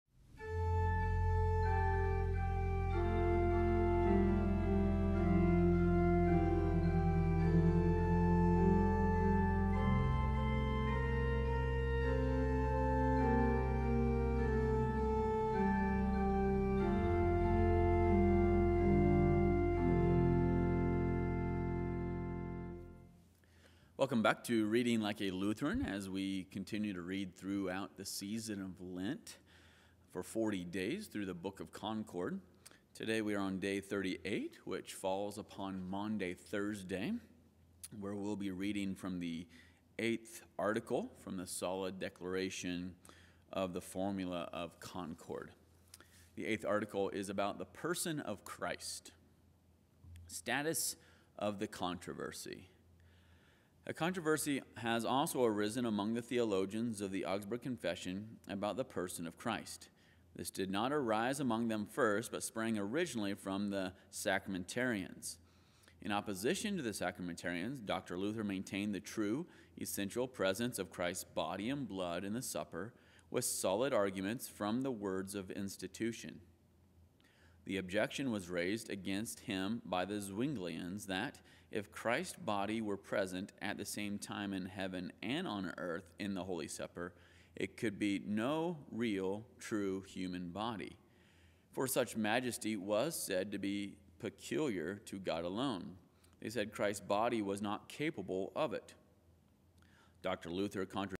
Day 38 - 40 Days Reading Like a Lutheran - The Book of Concord
Day 38 - Maundy Thursday - Formula of Concord: Solid Declaration, Article VIII.